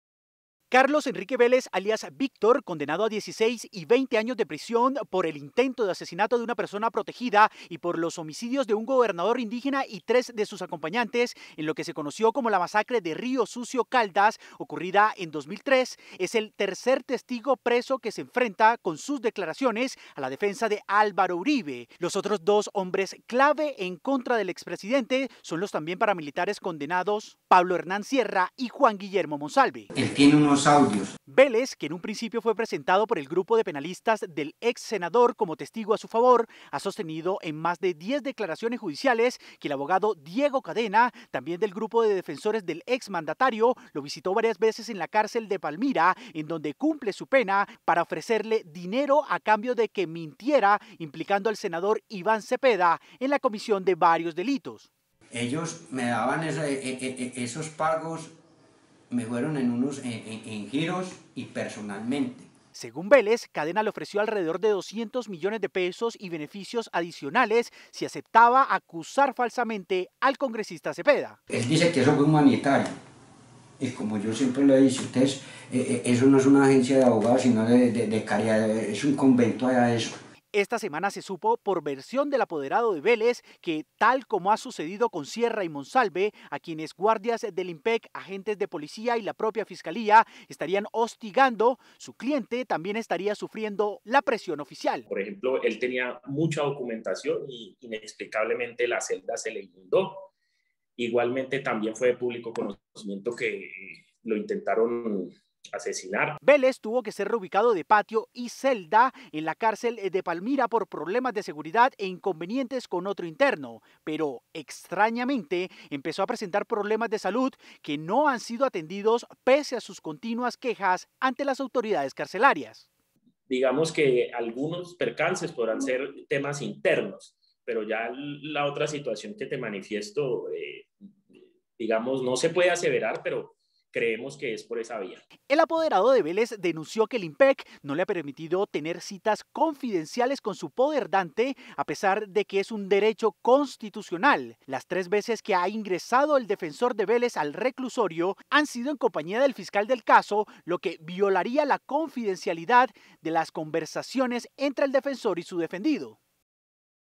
Abogado de testigo Vélez que declaró contra Cadena, insiste en que lo están hostigando
Desde que la Fiscalía asumió la investigación contra Álvaro Uribe, entre otras por el presunto soborno a Carlos Enrique Vélez, la situación de él se ha complicado en la cárcel: no le permiten hablar con su abogado, una inundación estuvo a punto de dañar sus papeles y se ha sentido mal de salud últimamente.